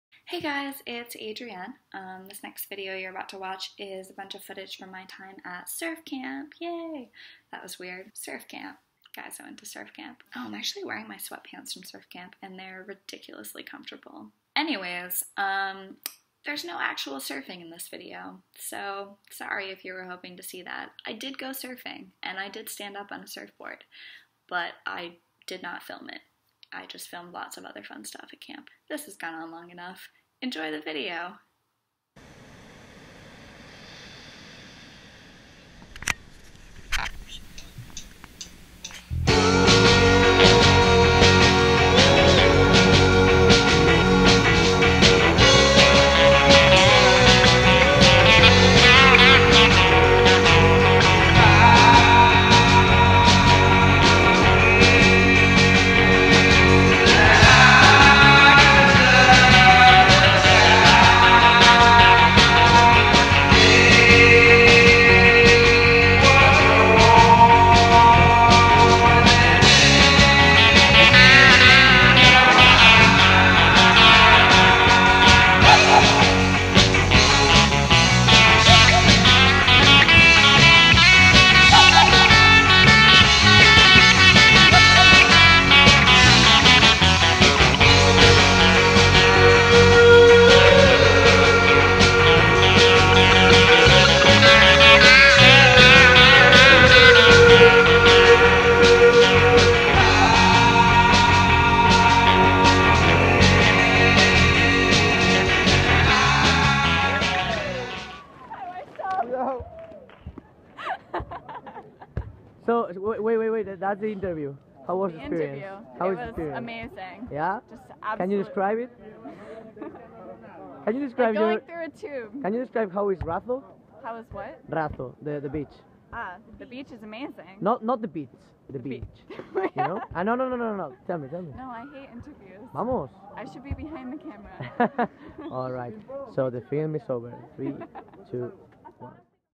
Surf Camp in Spain (Adrienne's Amateur Adventures: Galicia, Spain)
I took a break from the harsh backpacker's life this August to go to surf camp for a week in Carballo, Galicia, Spain. It was an amazing week - the surfing was great, the people were awesome, and the location was perfect! I hope you enjoy the video of some of the fun stuff we got up to at camp, and if you want to read more about my time there, check out my blog post